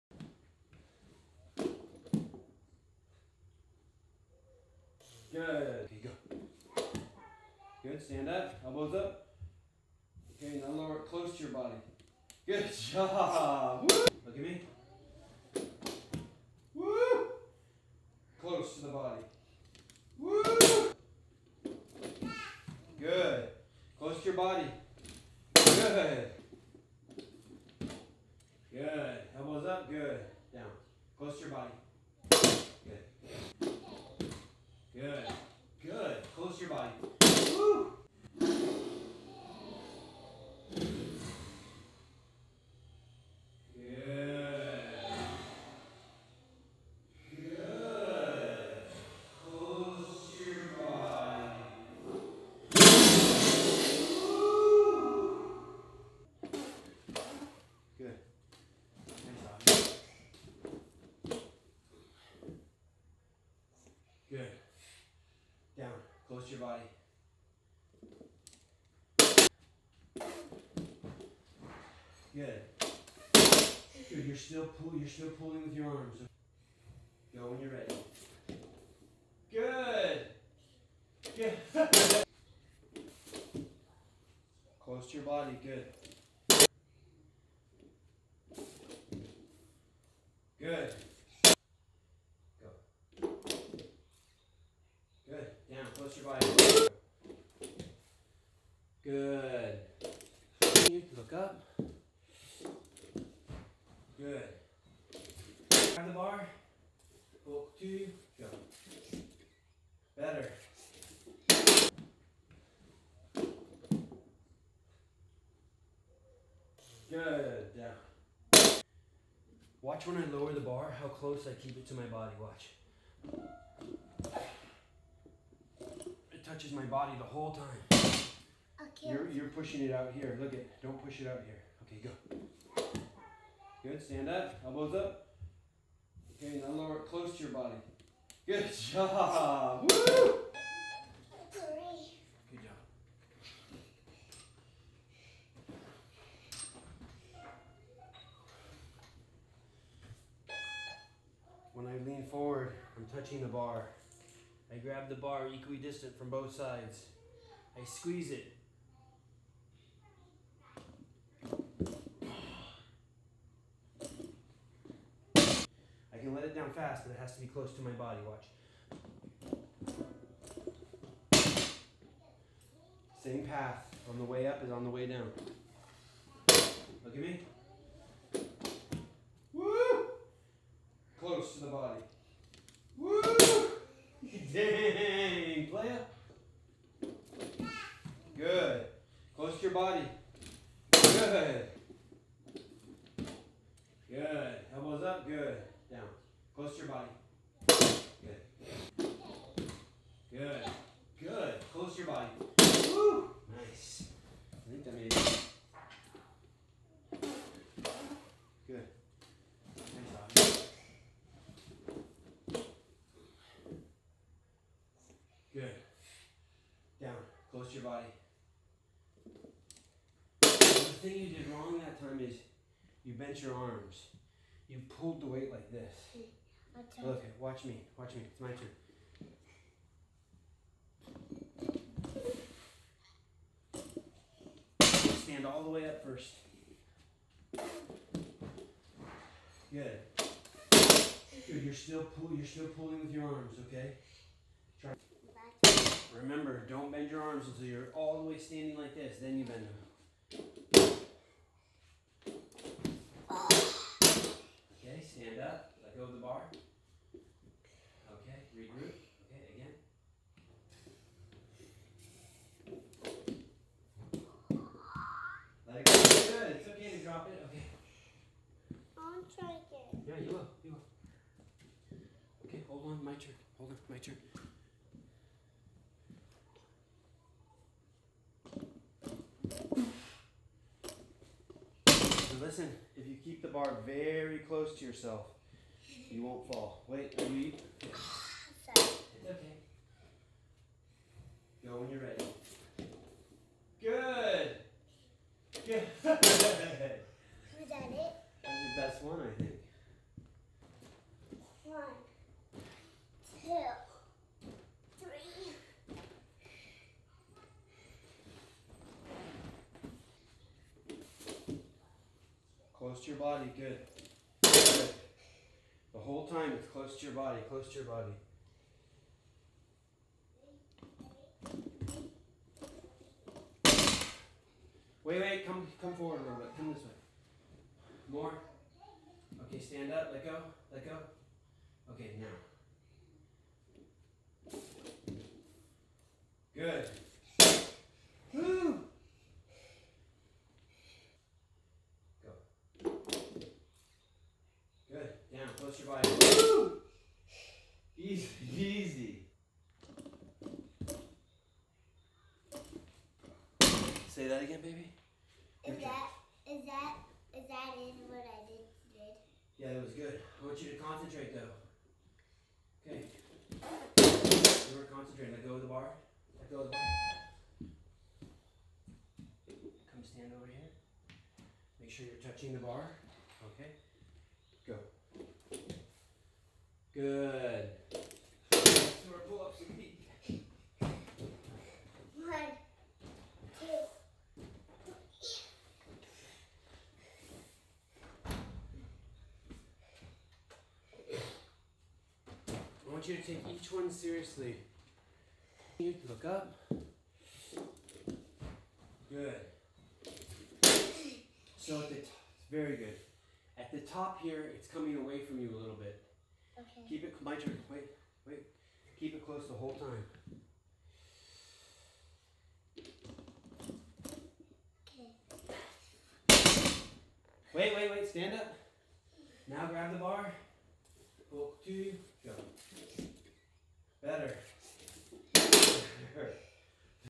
Squat Cleaning 5 year old
Learning the Squat Clean at 5 years old